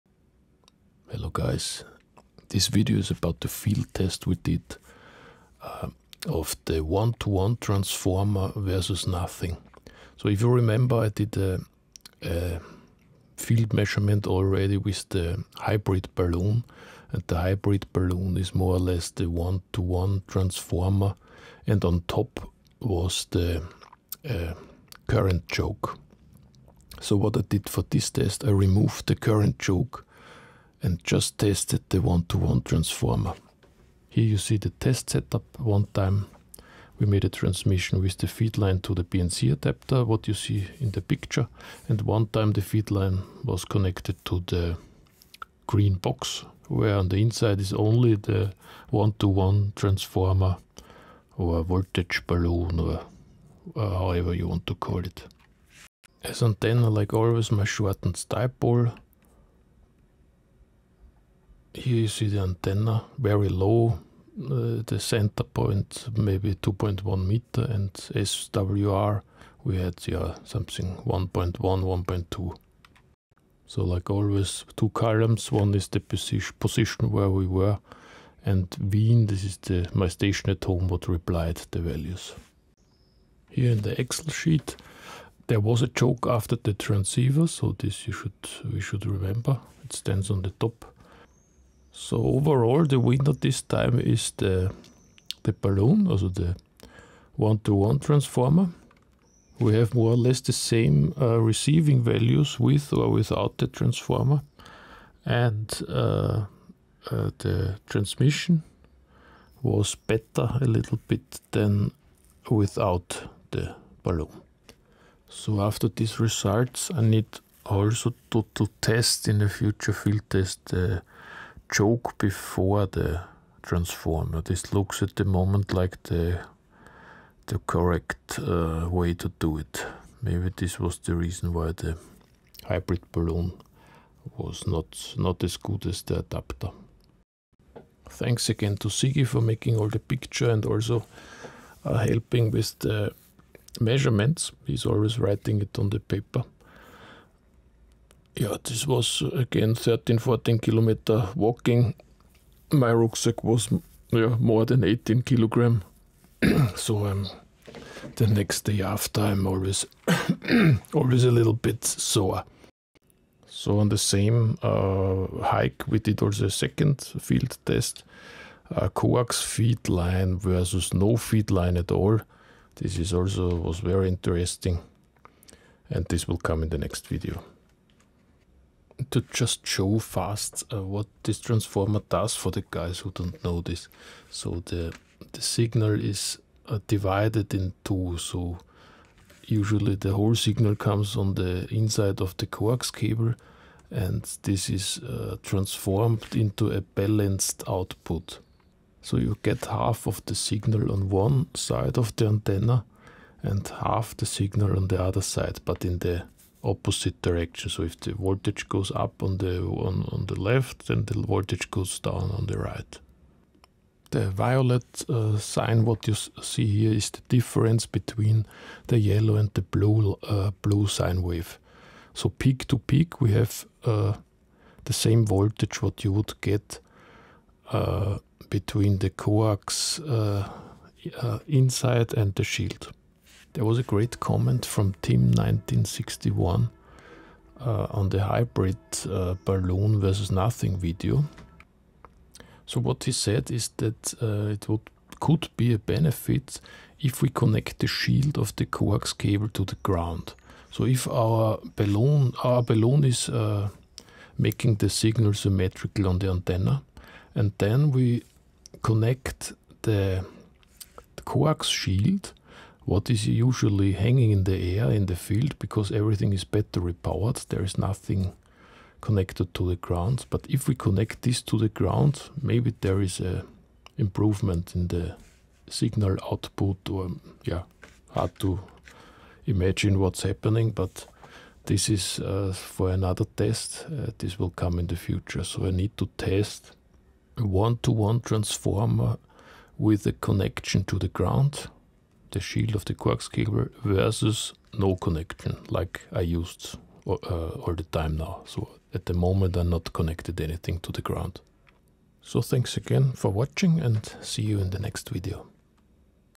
NVIS Dipole, 1:1 Transformer vs Nothing, Field measurements
Field measurements of a 1:1 transformer against nothing.
Using digital modes to compare two setups in the field.